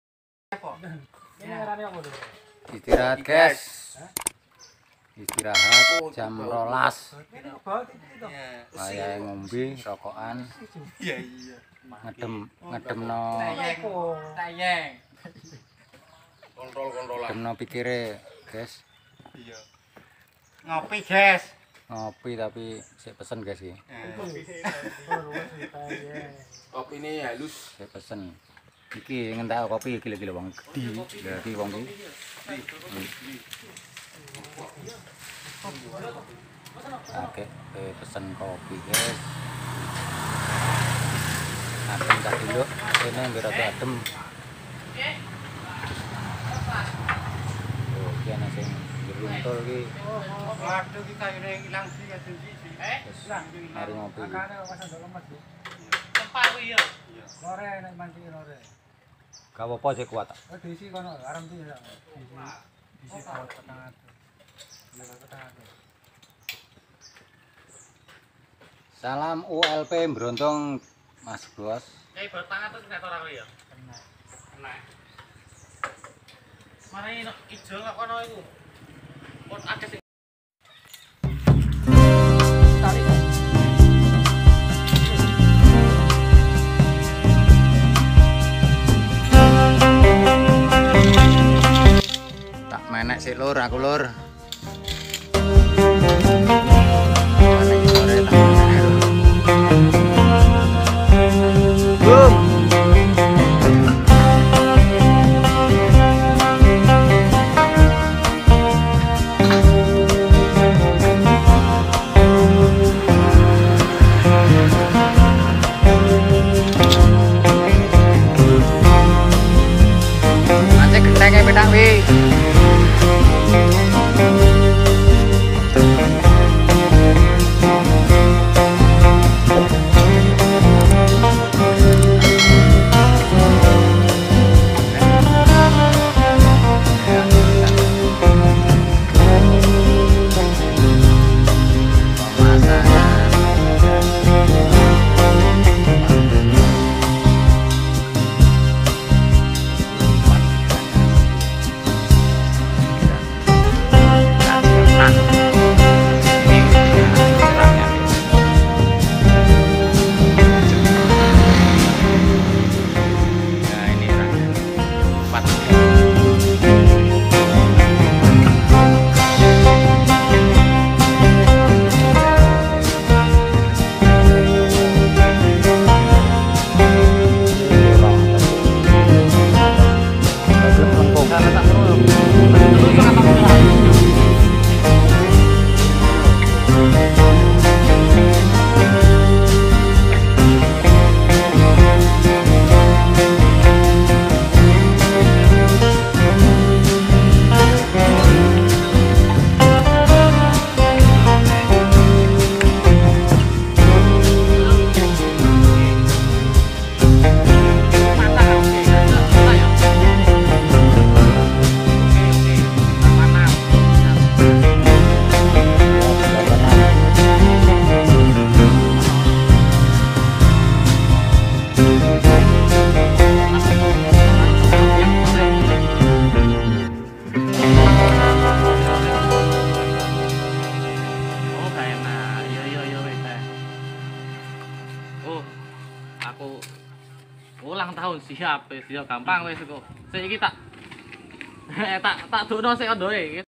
@japlektv(part2) PEMASANGAN WESPEK PADA JARINGAN LISTRIK PLN
pemasangan dan perbaikan jaringan listrik PLN ULP brondong tidak lupa juga saya ucapkan terima kasih kepada teman teman YouTuber semua atas dukungan nya sobatku